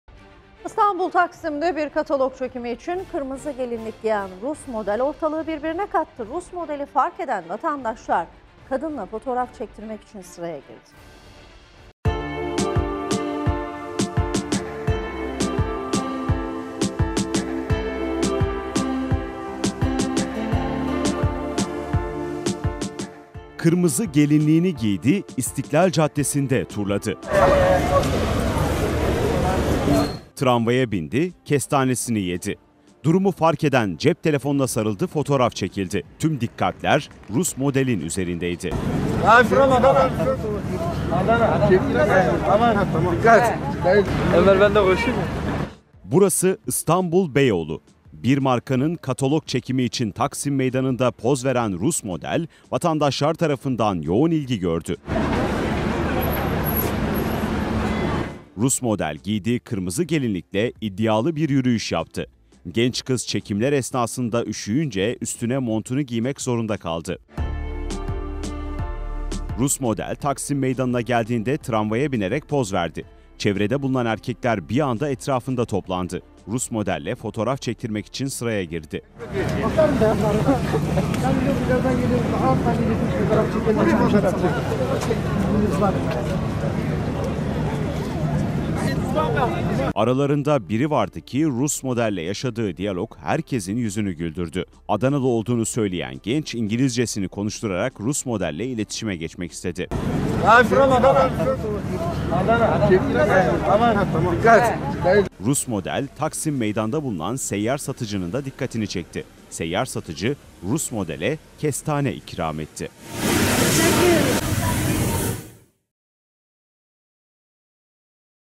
Rus Model Taksim'de İlgi Odağı Oldu
İstanbul Taksim'de bir katalog çekimi için kırmızı gelinlik giyen Rus model ortalığı birbirine kattı. Rus modeli fark eden vatandaşlar kadınla fotoğraf çektirmek için sıraya girdi.